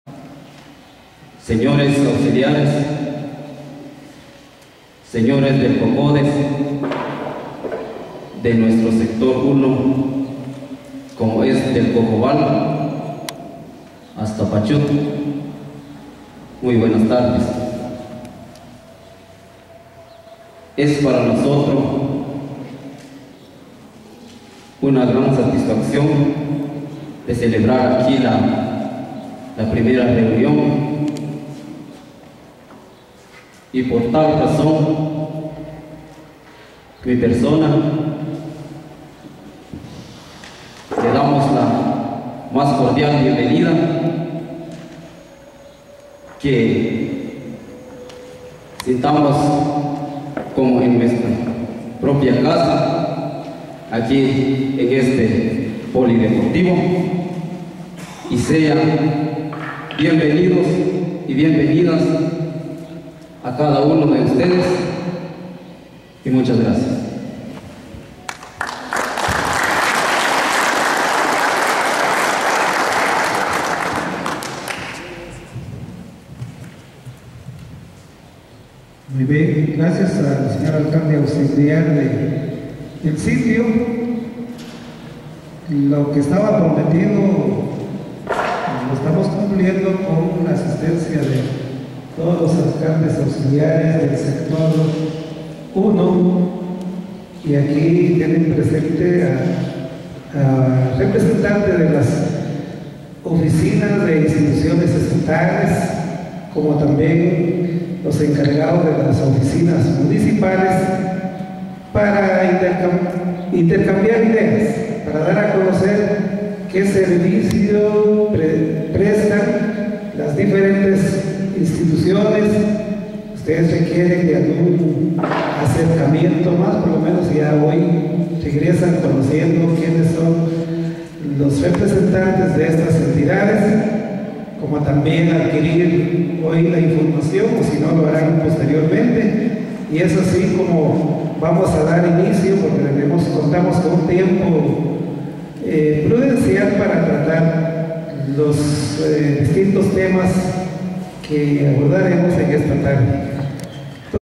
Bienvenida por Autoridad Comunitaria
Patzún, Bienvenida por Autoridad Comunitaria en la reunión de Autoridades Comunitarias en El Sitio